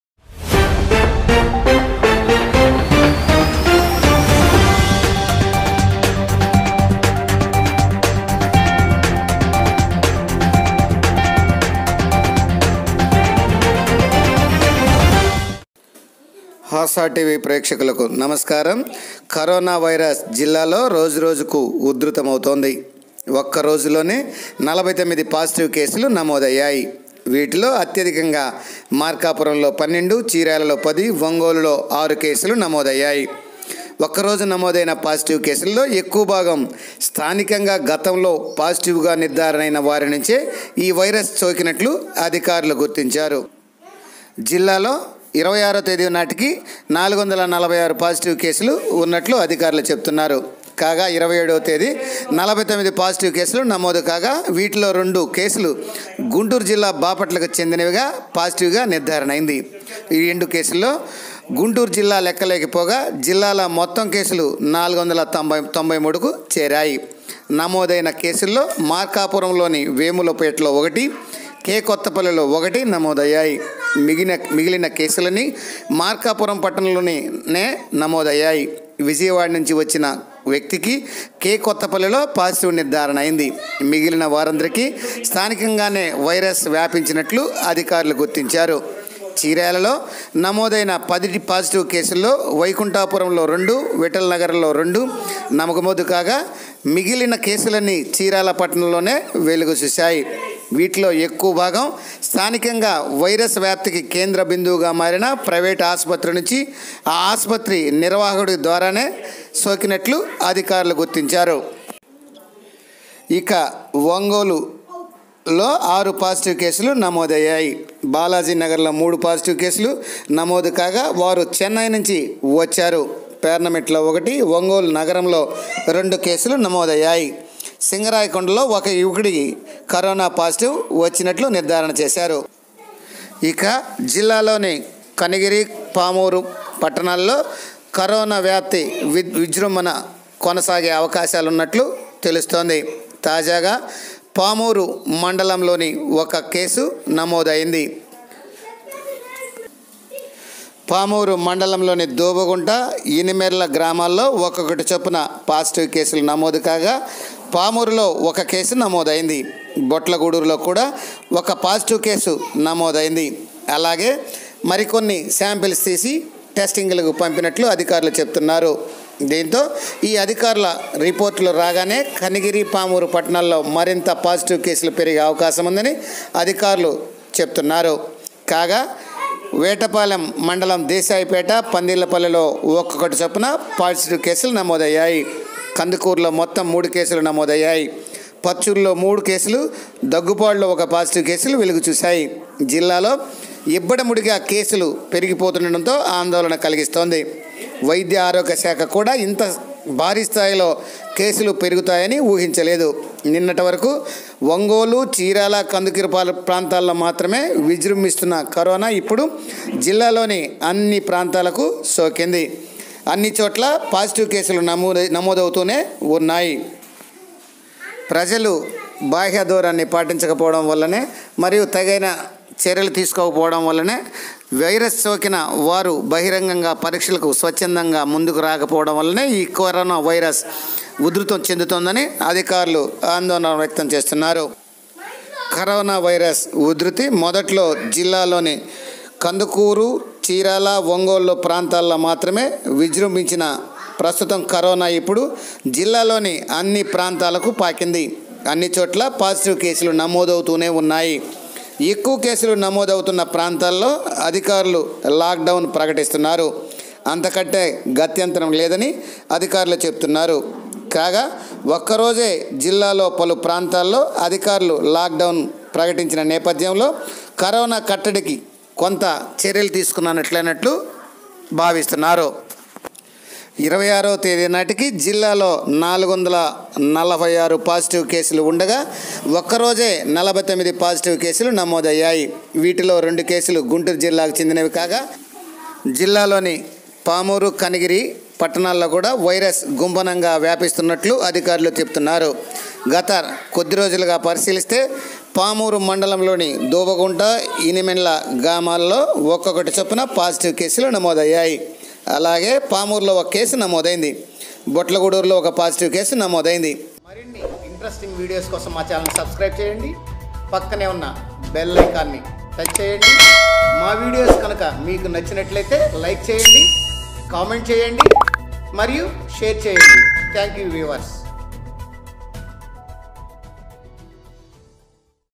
పెరుగుతున్న కరోనా కేసులు, కనివిరి,పామూరు లలో బిక్కుబిక్కు.
Increasing corona cases across the prakasam district